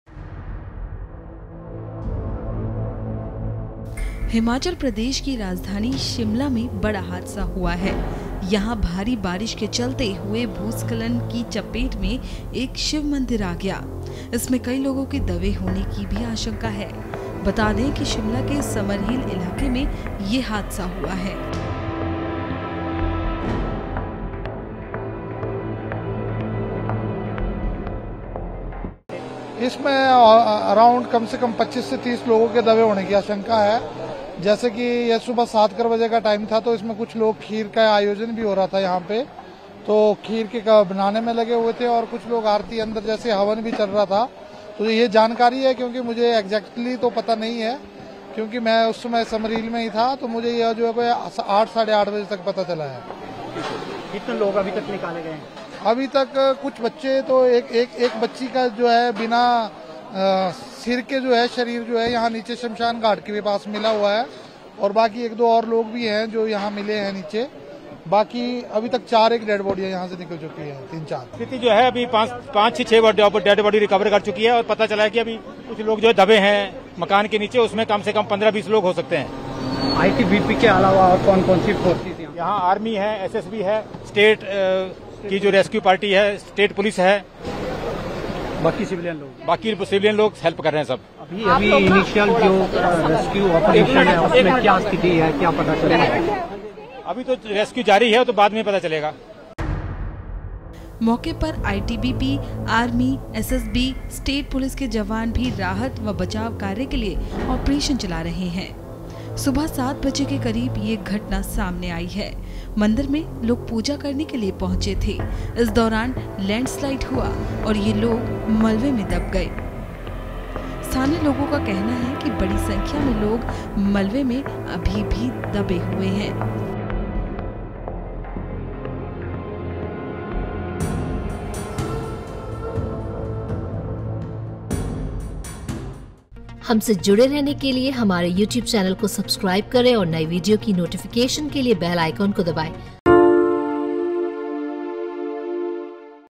Himachal Pradesh में बारिश से हाहाकार, शिव मंदिर ढहा, अब तक 41 की मौत
हिमाचल प्रदेश में बारिश से हाहाकारशिमला में भूस्खलन से मलबे में दबा शिव मंदिरहिमाचल में अब तक 41 लोगों की मौतCM ने आपात बैठक बुलाईराजभवन में 15 अगस्त का कार्यक्रम रद्द